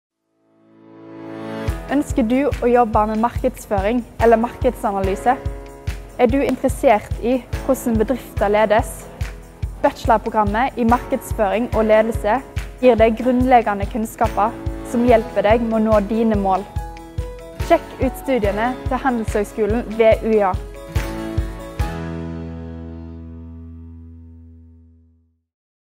Bachelor i markedsføring og ledelse
Ønsker du å jobbe med markedsføring, merkevareledelse eller markedsanalyse? Er du interessert i hvordan bedrifter ledes? Har du ambisjoner om på sikt å lede en markeds- eller salgsavdeling? Bachelorprogrammet i markedsføring og ledelse gir deg grunnleggende kunnskaper som hjelper deg å nå dine mål.